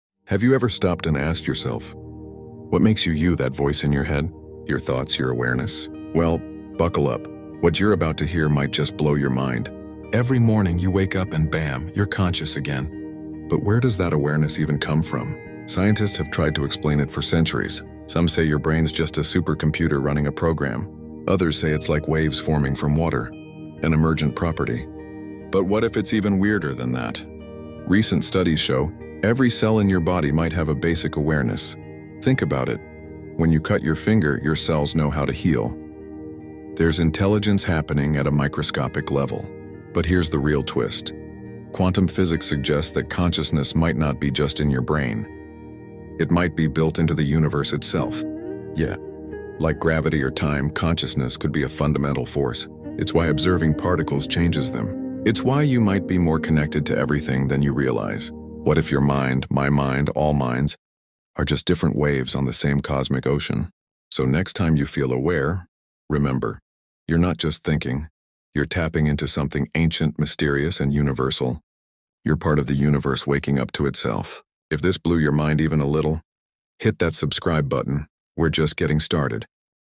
Understanding Consciousness: Origin Revealed
Explore the mysteries of consciousness and discover the origin of human awareness. In this thought-provoking video, we delve into the realms of neuroscience, philosophy, and psychology to understand the intricacies of consciousness. From spiritual awakening to mindfulness and personal growth, we examine the complex relationship between the brain and the mind. Join us on a journey to uncover the secrets of consciousness and gain a deeper understanding of the human experience. Whether you're interested in philosophy, psychology, or simply seeking a deeper understanding of yourself, this video is for you. So, sit back, relax, and embark on a fascinating exploration of the human consciousness.